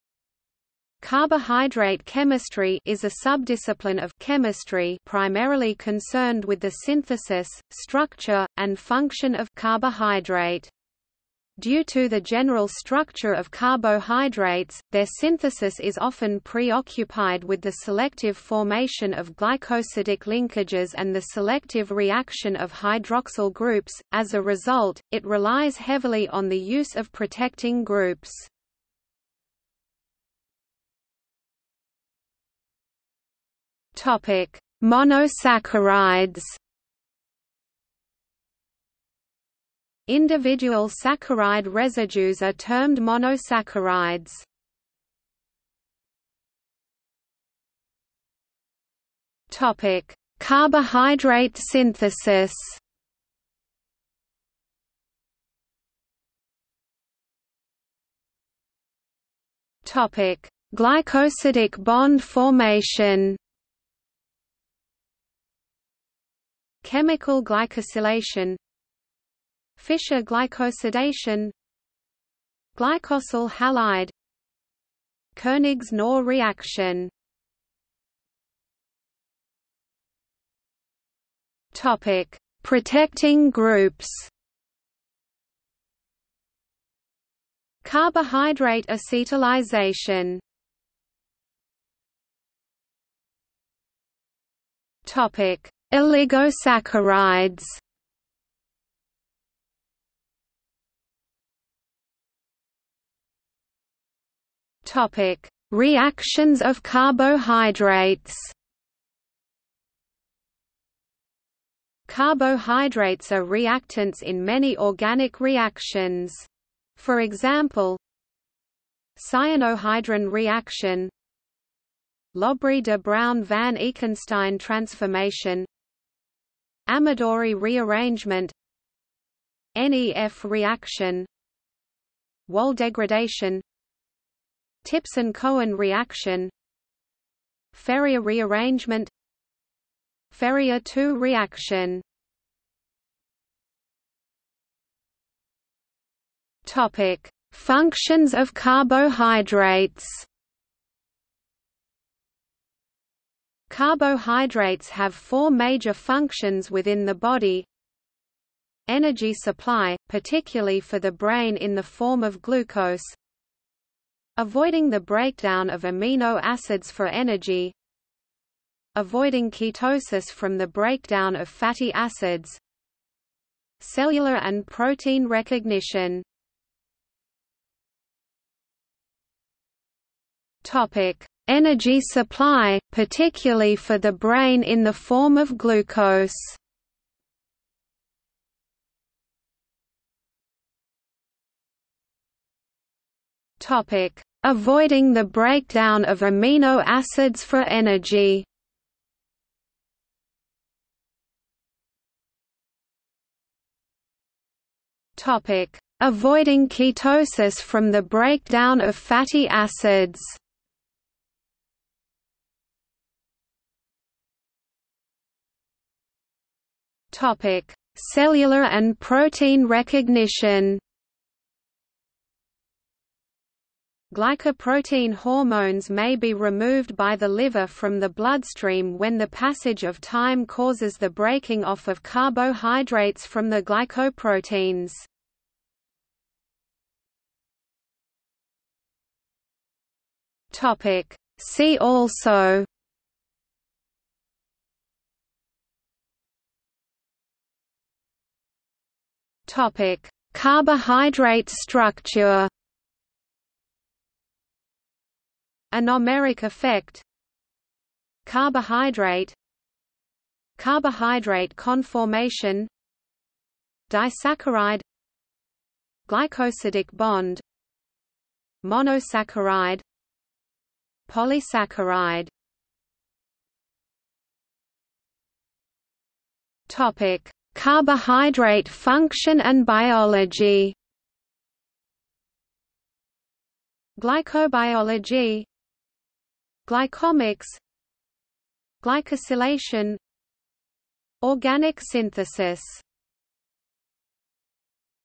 Carbohydrate chemistry | Wikipedia audio article
00:00:30 1 Monosaccharides
00:00:46 2 Carbohydrate synthesis
00:00:56 3 Glycosidic bond formation
00:01:18 4 Protecting groups
00:01:31 5 Oligosaccharides
00:01:41 6 Reactions of carbohydrates
00:02:23 7 Functions of carbohydrates
00:02:57 7.1 Energy supply, particularly for the brain in the form of glucose
00:03:10 7.2 Avoiding the breakdown of amino acids for energy
00:03:21 7.3 Avoiding ketosis from the breakdown of fatty acids
00:03:33 7.4 Cellular and protein recognition
00:03:57 8 See also
00:04:06 8.1 Carbohydrate structure
00:04:33 8.2 Carbohydrate function and biology

- Socrates

SUMMARY
'Carbohydrate chemistry' is a subdiscipline of [chemistry] primarily concerned with the synthesis, structure, and function of [carbohydrate]. Due to the general structure of carbohydrates, their synthesis is often preoccupied with the selective formation of glycosidic linkages and the selective reaction of hydroxyl groups; as a result, it relies heavily on the use of protecting groups.